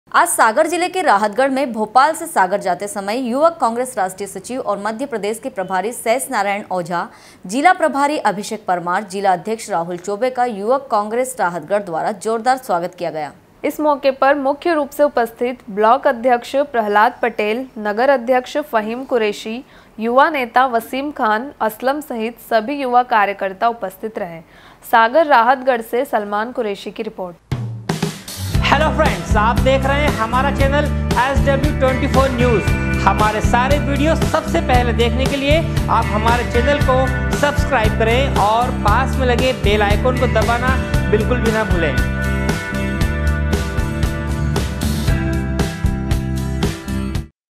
rahatgarh- आज सागर जिले के राहतगढ़ में भोपाल से सागर जाते समय युवक कांग्रेस राष्ट्रीय सचिव
आज सागर जिले के राहतगढ़ में भोपाल से सागर जाते समय युवक कांग्रेस राष्ट्रीय सचिव और मध्य प्रदेश के प्रभारी शेष नारायण ओझा जिला प्रभारी अभिषेक परमार जिला अध्यक्ष राहुल चैबे का युवक कांग्रेस राहतगढ़ द्वारा जोरदार स्वागत किया गया। सागर राहतगढ़ से सलमान कुरेशी की रिपोर्ट